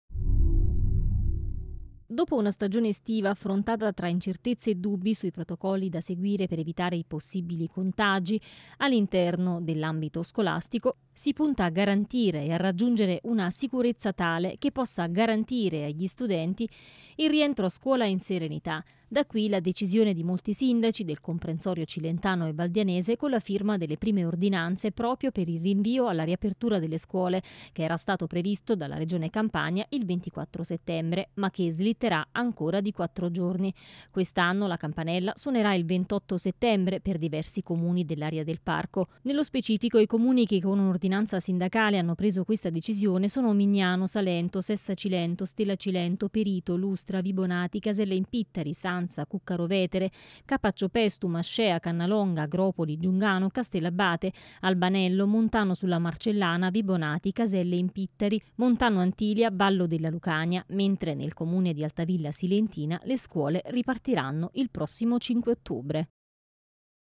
Scuola, Per diversi comuni dell'area parco la campanella suonerà il 28 Settembre.
Dopo una stagione estiva affrontata tra incertezze e dubbi sui protocolli da seguire per evitare possibili casi di contagio all'interno delle scuole, si punta a garantire e a raggiungere una sicurezza tale che possa garantire agli studenti il rientro a scuola in serenità.Da qui la decisione di molti sindaci del comprensorio cilentano e valdianese con la firma delle prime ordinanze proprio per il rinvio alla riapertura delle scuole che era stato previsto dalla regione Campania il 24settembre, ma che slitterà ancora di 4 giorni.